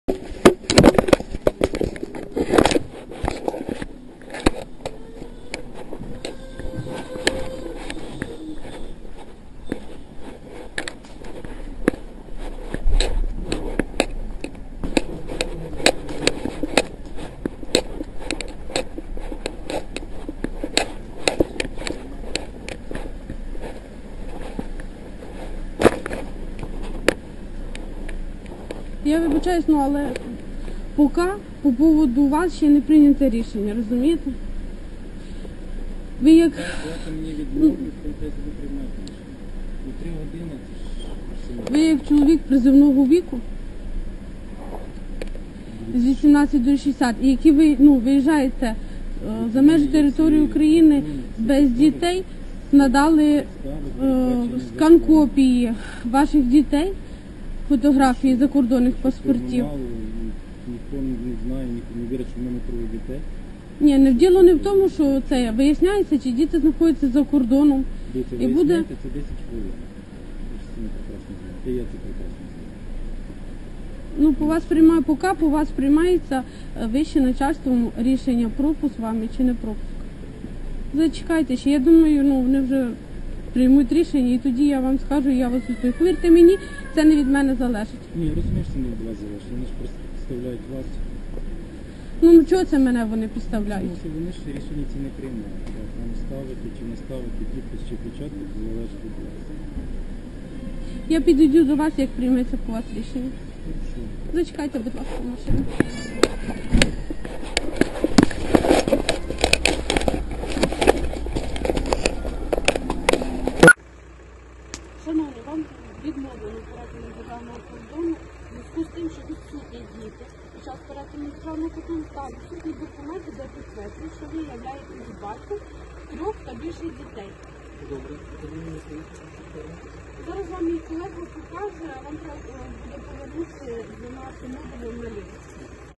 Відео з прикордонного пункту за участю екс-прокурора Руслана Рябошапки.
З'явилося відео з прикордонного пункту за участю екс-прокурора Руслана Рябошапки. Він хотів потрапити з України до Угорщини, прикриваючись дітьми.

"Вам відмовлено у перетині державного кордону, оскільки з вами немає дітей та відсутні документи, де ви є батьком трьох і більше дітей", - заявили в Держприкордонслужбі.

🇺🇦 Актуальні новини сьогодення.